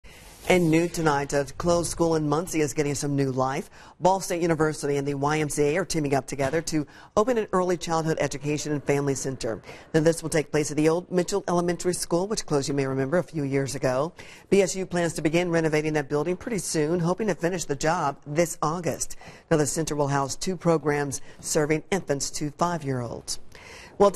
Old Muncie School Turned Into Education Center
A closed school in Muncie is getting new life. Ball State and the YMCA are teaming up to open an early childhood education and family center. This will be at the old Mitchell Elementary School which closed a few years ago.